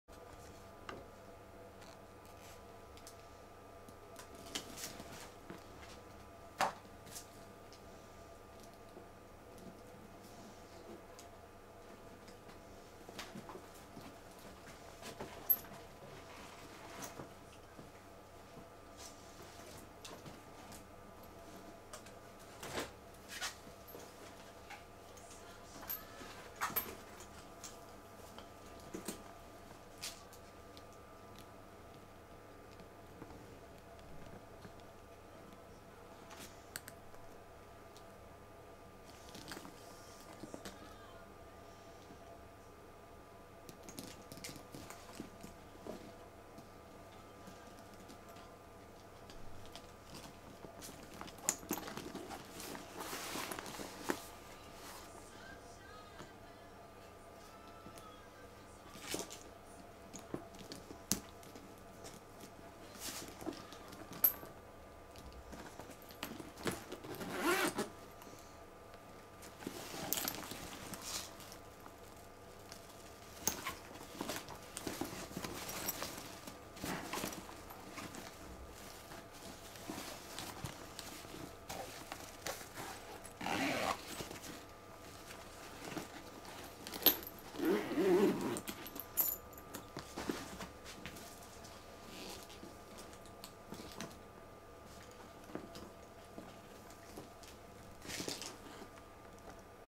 Сумка женская Marino Orlandi 3822
ткань, Италия
Размеры: 35х29х14 см, высота ручек 16 см. Внутри карман на молнии и карман для мобильного. Сзади карман на молнии.